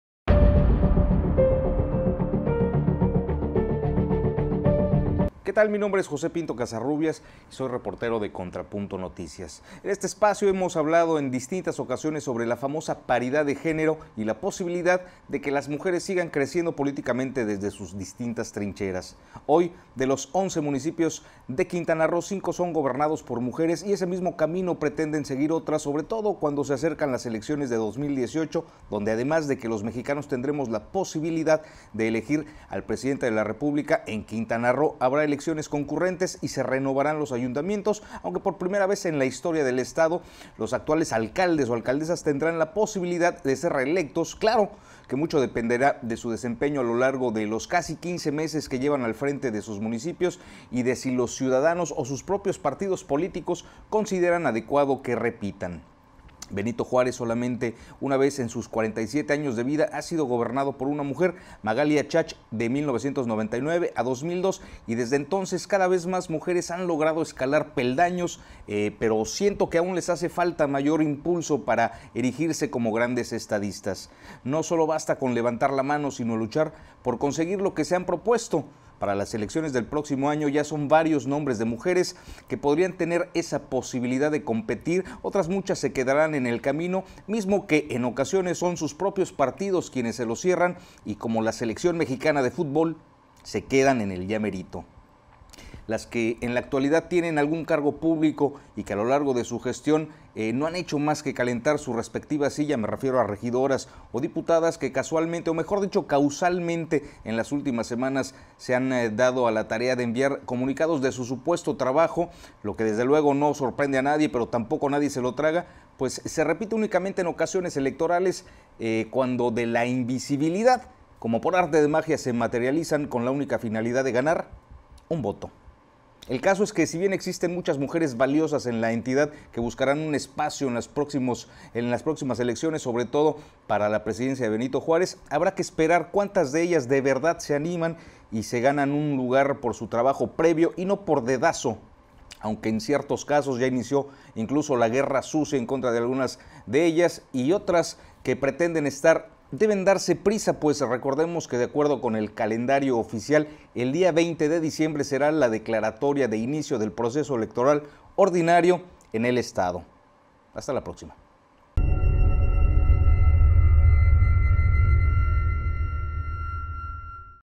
El papel de la mujer en política
Columna de José Pinto Casarrubias

En la actualidad muchas féminas tienen algún cargo público y, a lo largo de su gestión no han hecho más que calentar su respectiva silla, -me refiero a regidoras o diputadas- que casualmente o mejor dicho causalmente, en las últimas semanas se han dado a la tarea de enviar comunicados